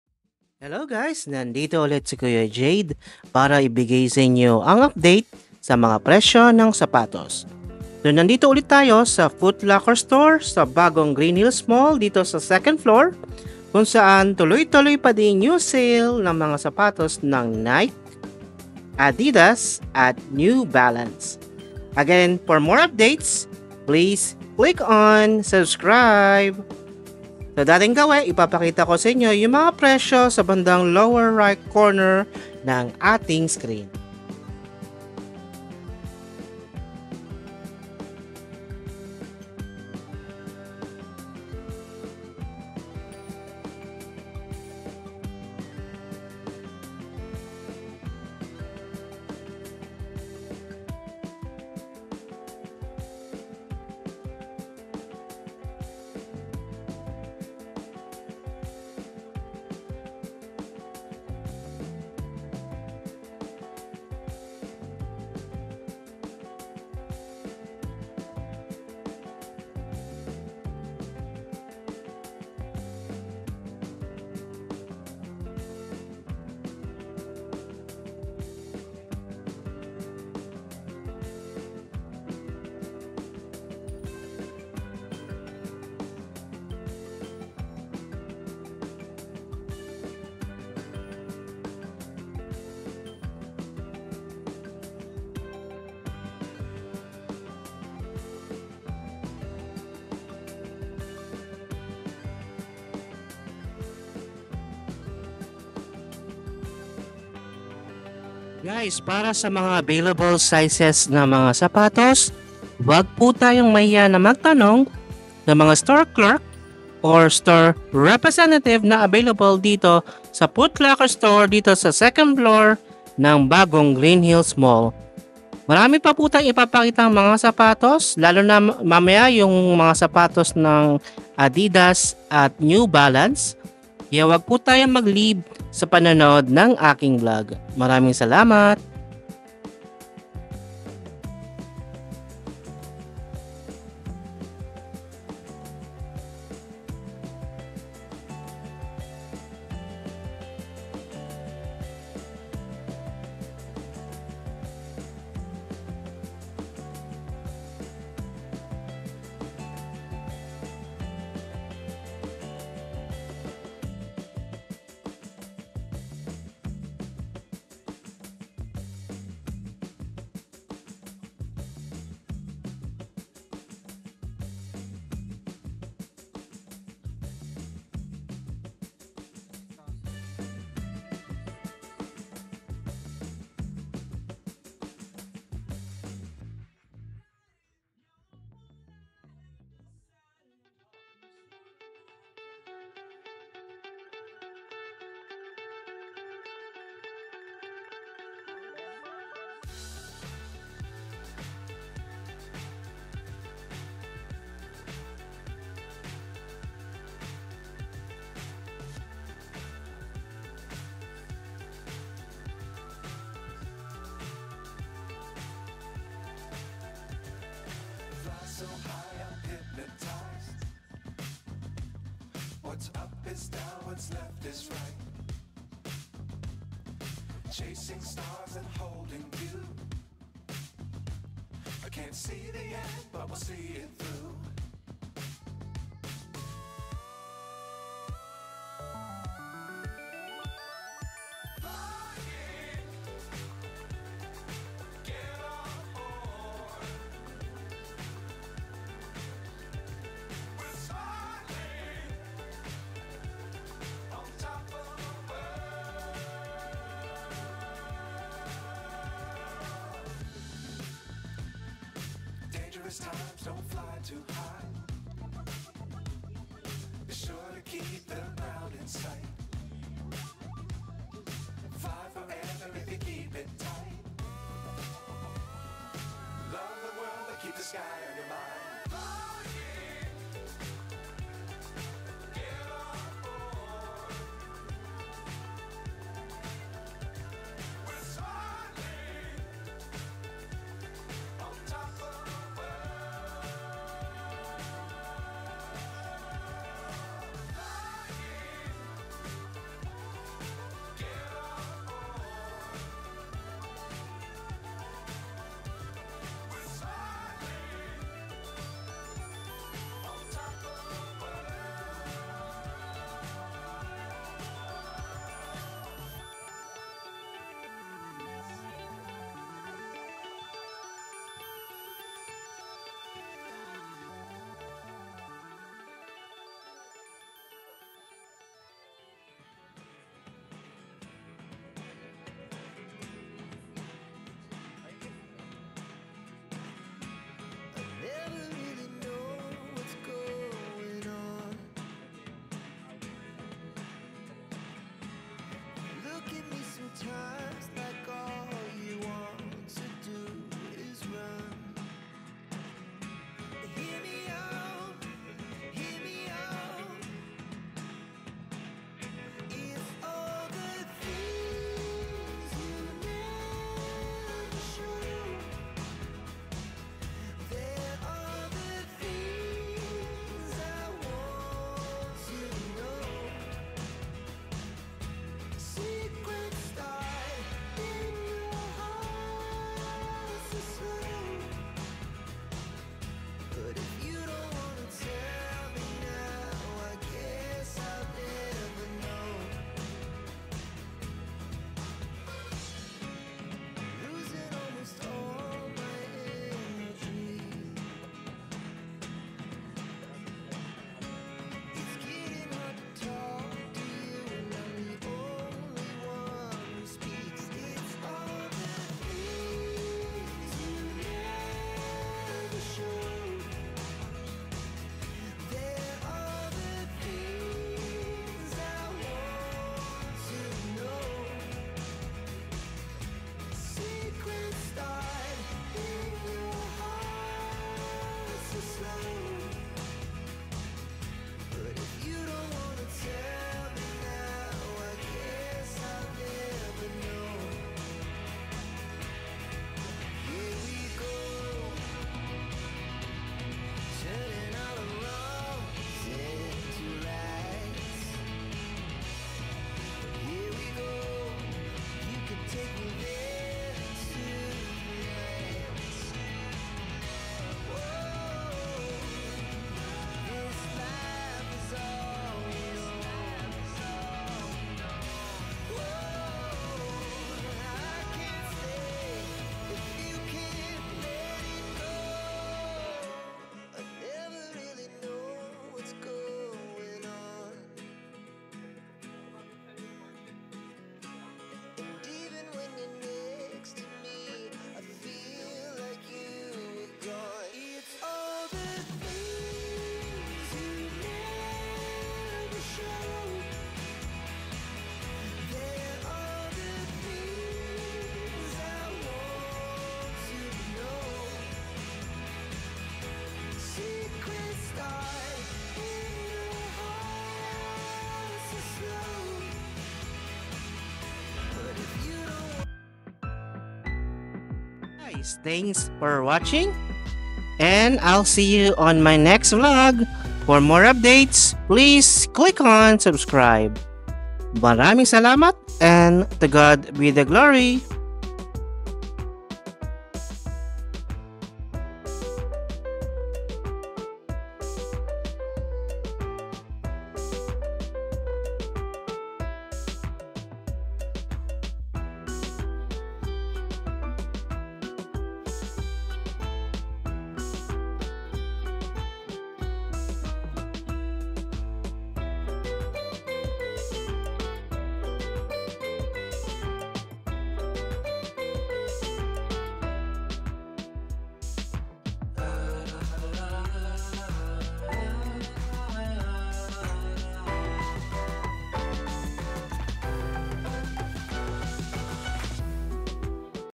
TULOY TULOY PA RIN ANG SALES NG NIKE, ADIDAS AT NEW BALANCE SHOES SA FOOTLOCKER GREENHILLS
Nagvisit muli tayo sa Footlocker Store sa New Greenhills Mall dahil tuloy tuloy pa rin ang kanilang Sale sa mga sapatos ng Nike, Adidas at New Balance.

Store Location:
2nd Level, New Greenhills Mall, Ortigas Avenue, Greenhills, San Juan City

Ang Price ng mga sapatos ay maaring magbago from time to time.
Please inquire tayo sa store clerk para sa mga available sizes ng mens at womens shoes sa time ng ating pagvisit.

Jed Bello Nepuncio